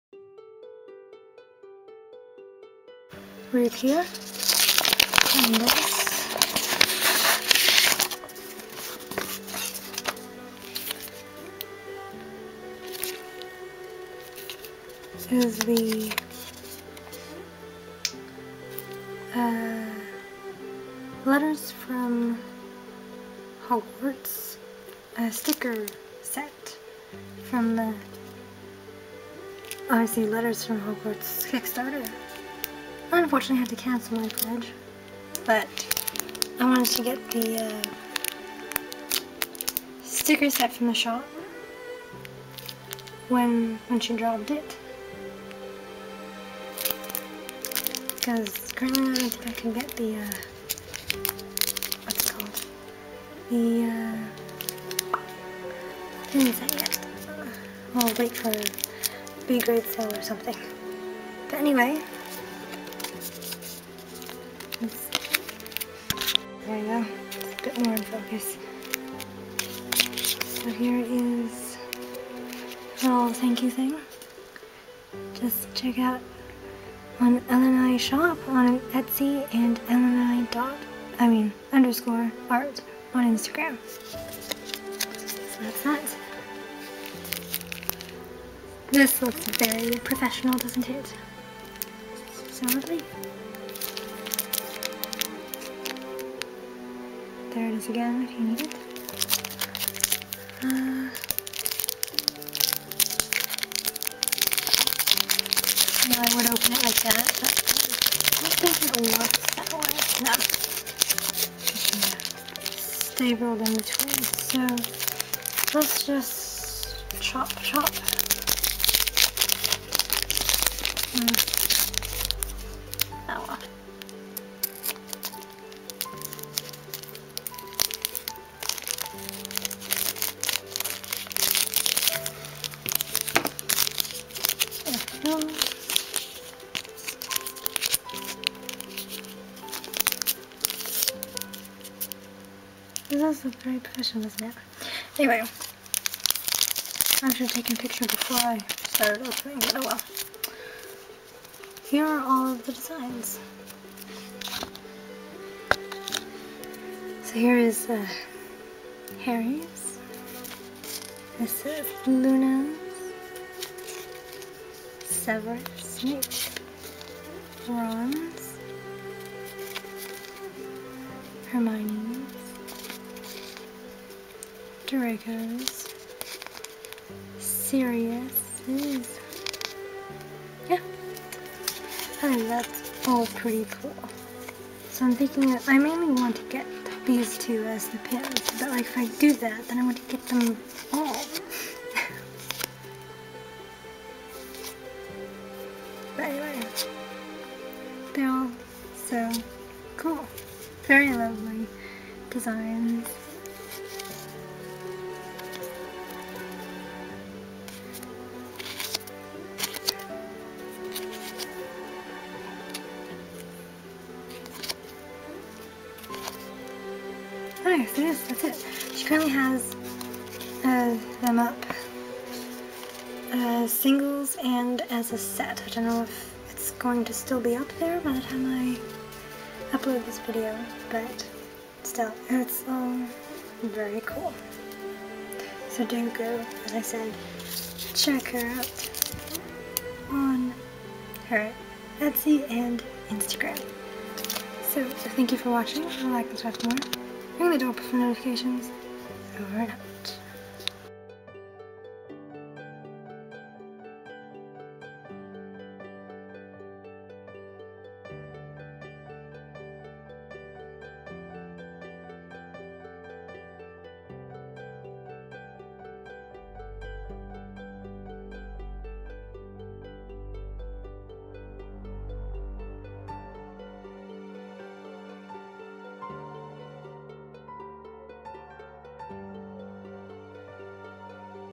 Letters From Magic School Sticker Set! ~ starrypins
Opening of: Letters From Magic School Sticker Set! ~ elenai_art

pfwvssj